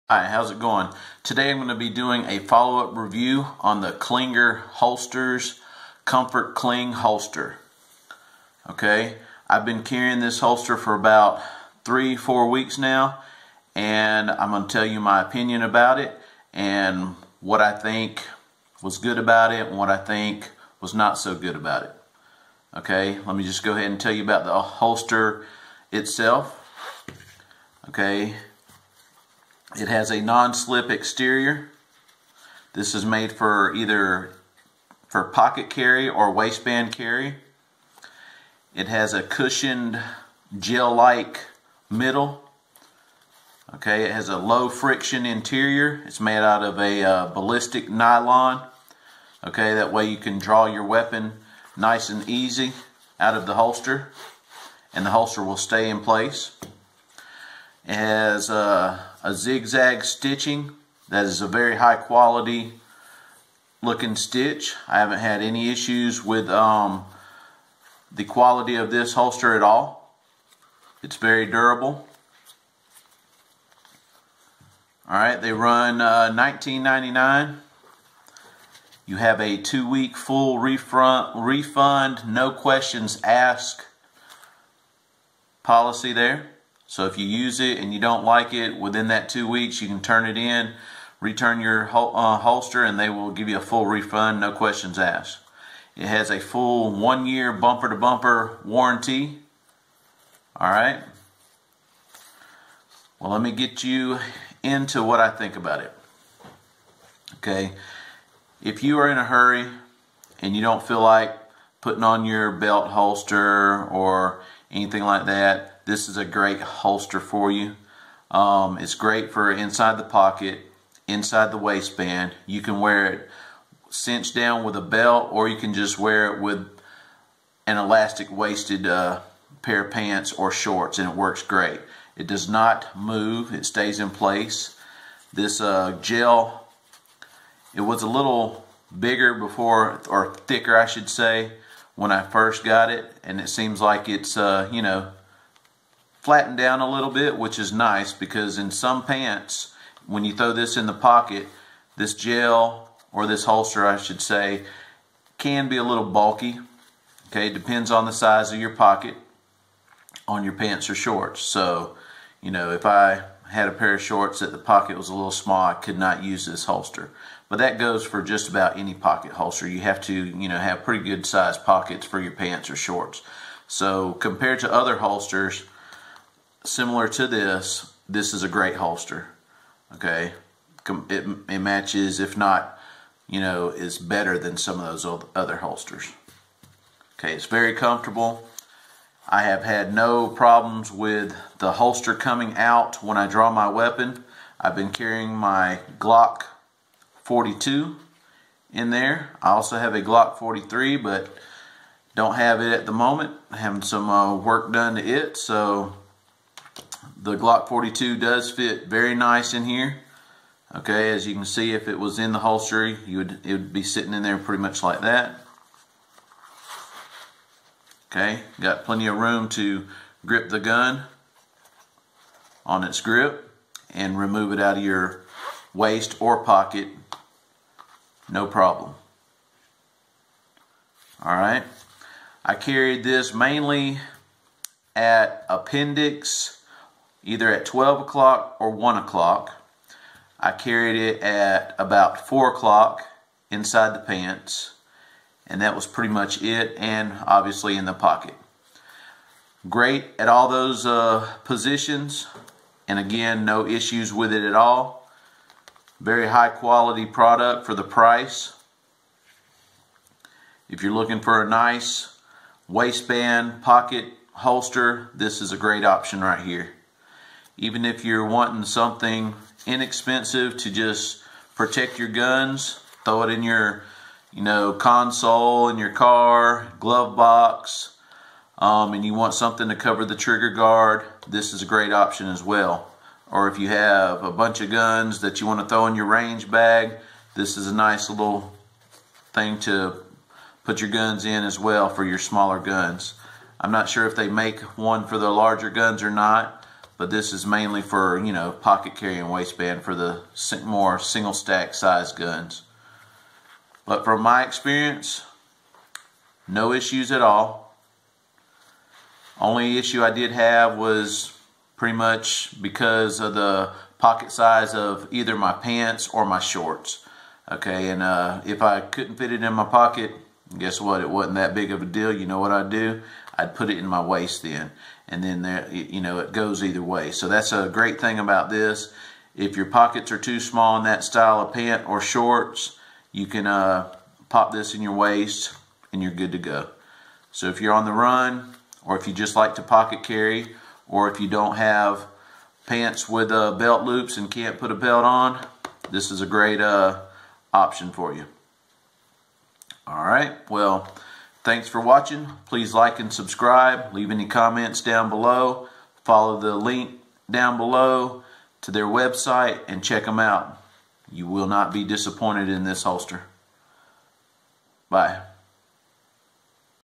Clinger Holsters Comfort Cling Holster Review
This holster is of high quality and is very comfortable. Priced at $19.99 you can't beat it for the versatility that it has. It worked great for me and definitely has it's place when it comes to conceal carrying.

One of the benefits of using a Comfort Cling holster is that it can be used for so many different gun models. In fact, most Single Stack pistols will fit into the Comfort Cling holster.

The following is the list of pistols known to fit into the Comfort Cling holster.

Fits Most Single Stack Pistols

1911 3″
1911 4″ *
1911 5″ *
Bersa BP9cc
Bersa Thunder 380
Boberg XR9-L
Browning Hi-Power *
Glock 43
HK P7M8
Kahr CM9
Kahr CM40
Kahr CT9
Kahr CT40
Kahr CT45
Kahr CW9
Kahr CW40
Kahr PM45
Kel-Tec P11
Kel-Tec PF9
Polish P-64
Remington R51
Ruger SR22
Ruger LC9
Ruger LC380
S&W Shield 9mm
S&W Shield 40
Sig P230
Sig P232
Sccy CPX-1
Sccy CPX-2
Springfield Armory XDs 3.3″
Springfield Armory XDs 4.0″ *
S&W M&P 22c
Taurus PT709
Taurus PT740
Taurus PT111 G2
Taurus PT140 G2
Walther CCP
Walther P22
Walther PPS M1
Walther PPS M2
Walther PK380

Guns marked with * will protrude past bottom of holster

Because the Comfort Cling fits so many gun models, it is great to have an extra one in the safe for a great backup holster.